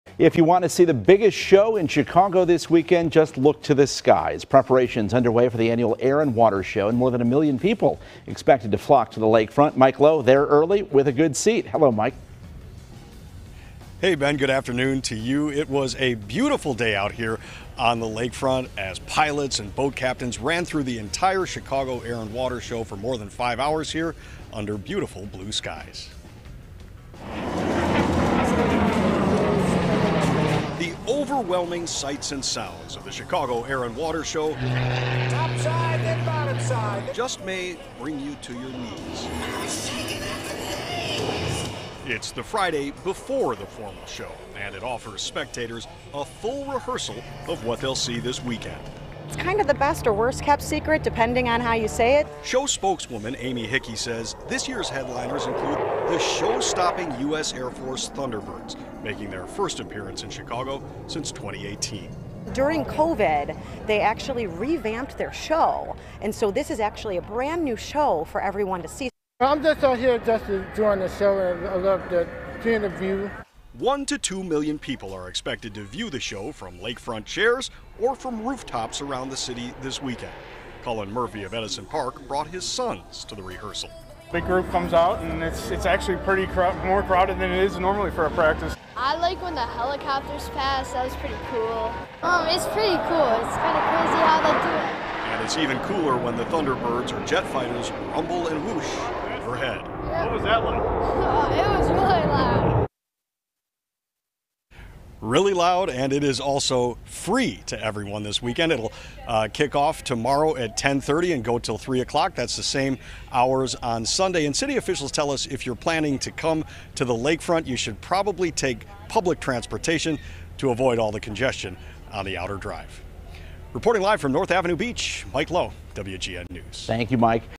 Chicago Air and Water Show 2023 kicks off over Lake Michigan
The Thunderbirds soared through the Windy City skies Friday morning, kicking off rehearsals for the Chicago Air and Water show this weekend.